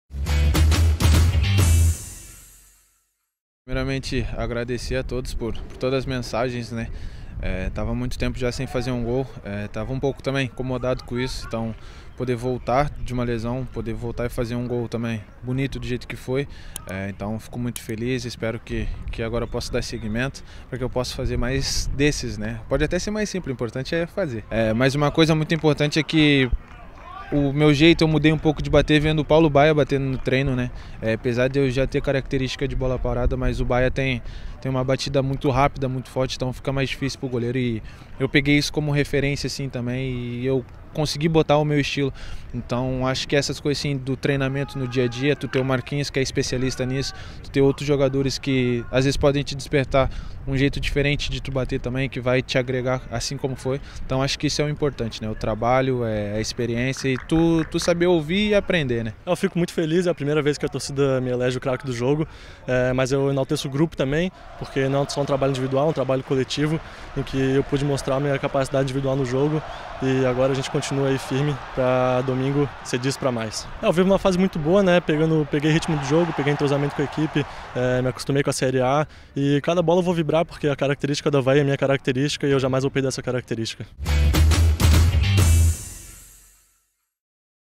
Na reapresentação do elenco, Jean Pyerre e Arthur Chaves comentaram sobre empate contra o Palmeiras
Vídeo  TV Avaí